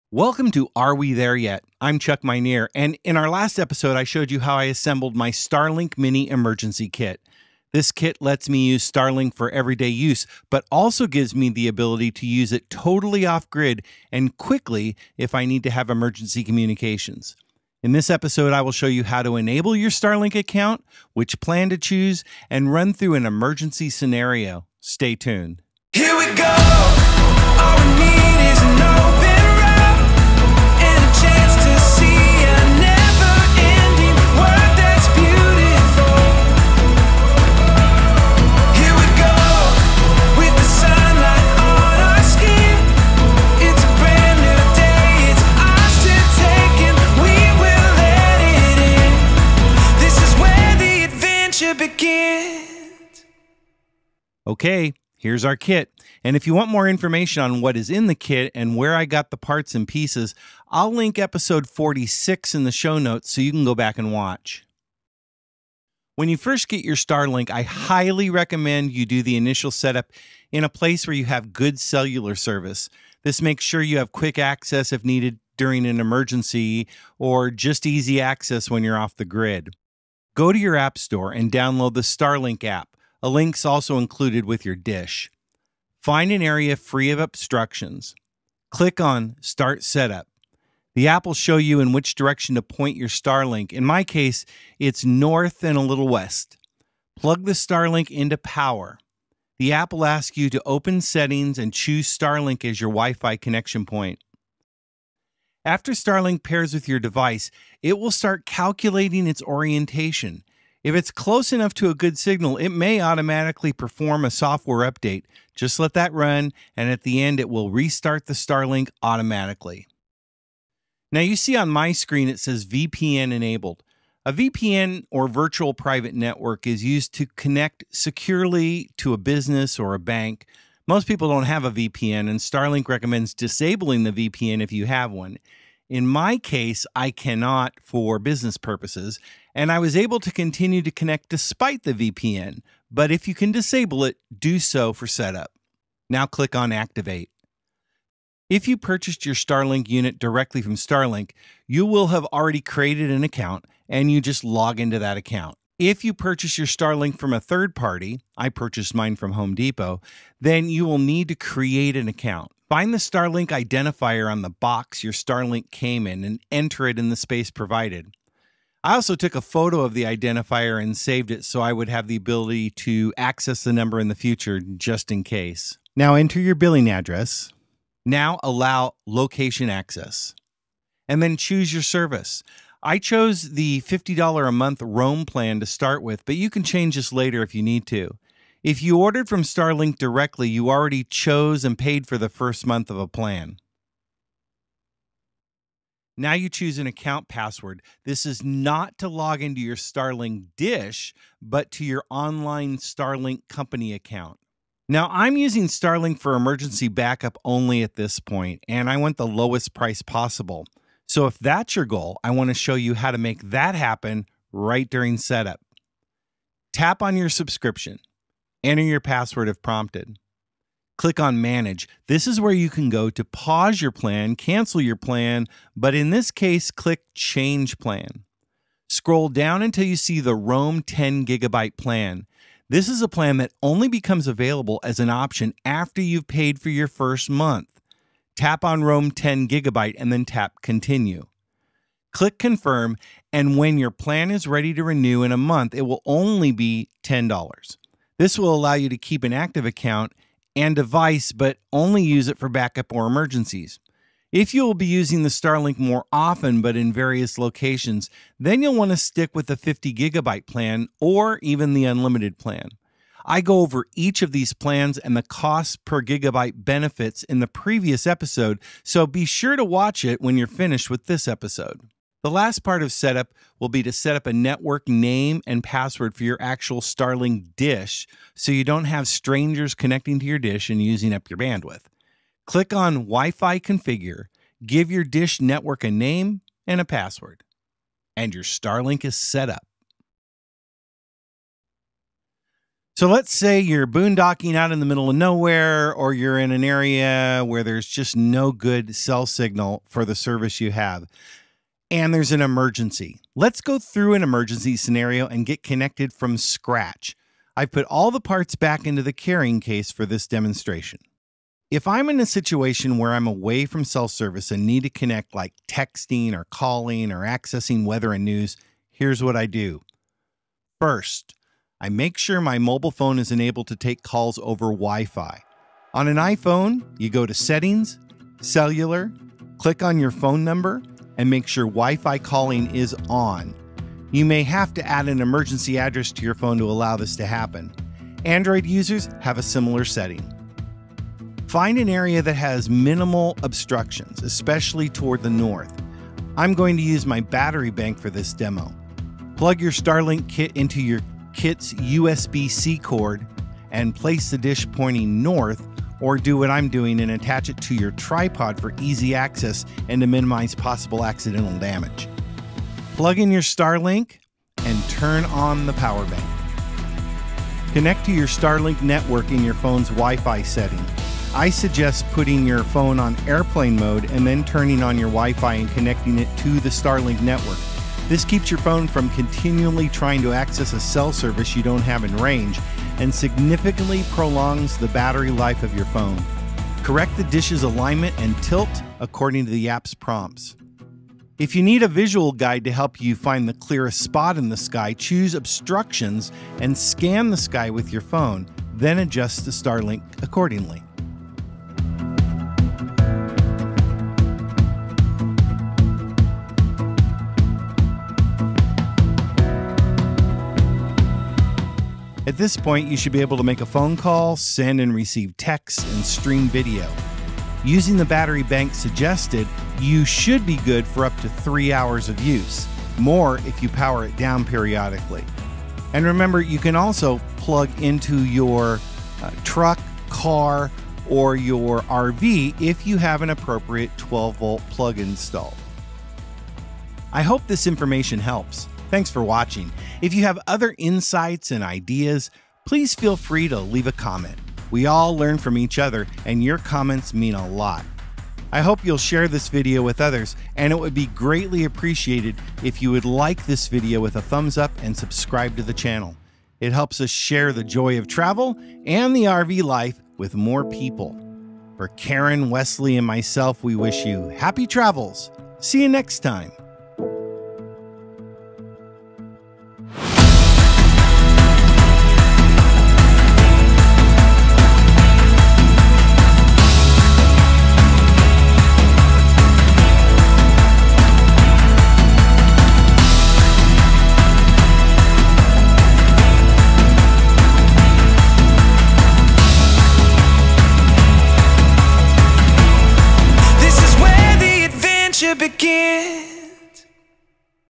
The Most Economical Way to Run your Starlink Mini: ready for any scenario - emergency & off grid
ℹ️ You've assembled your portable Starlink Kit from our last episode.  Now it's time to set up your account, create and secure your network, and start Starlink service.  In this episode I show you how to set up your kit, enable your account, get the lowest priced account possible, and run through an emergency scenerio.  If you want to see what is in the kit and how to assemble it, just watch episode 46.

00:00 Intro
00:54 Kit Overview
01:06 Initial Setup & Activation
03:25 Getting the Lowest Price Possible
04:15 Which Plan is Right for Me?
04:33 Network Name & Password
04:53 Emergency Scenario Run-through

Starlink Mini from Home Depot
Starlink Case
Starlink USB Cable
Starlink USB adapter
Starlink Mini Tripod Adapter
140 Watt Battery Bank
SAE to DC Barrel Adapter (RV direct power)
Starlink Mini Battery Adapters:

We are not RV experts and are not making recommendations. We share our opinions and what works for us, but you should do your own research and make your own decisions.